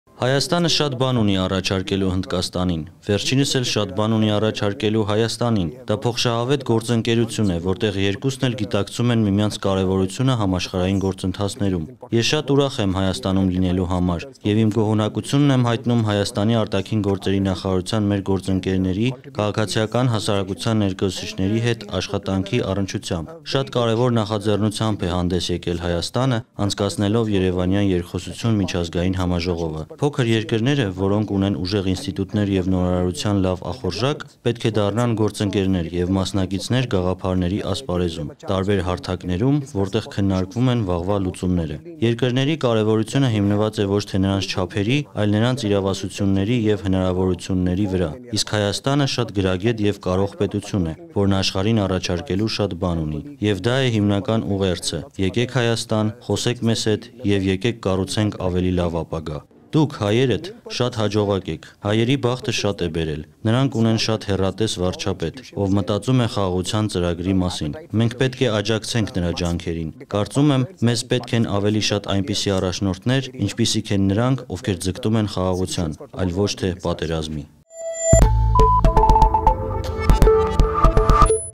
ՀՀ-ի ու Հնդկաստանի միջև փոխշահավետ գործընկերություն է հաստատվել․ փորձագետ
«Observer Research Foundation» կենտրոնի նախագահ Սամիր Սարան անդրադարձել է հայ-հնդկական հարաբերություններին։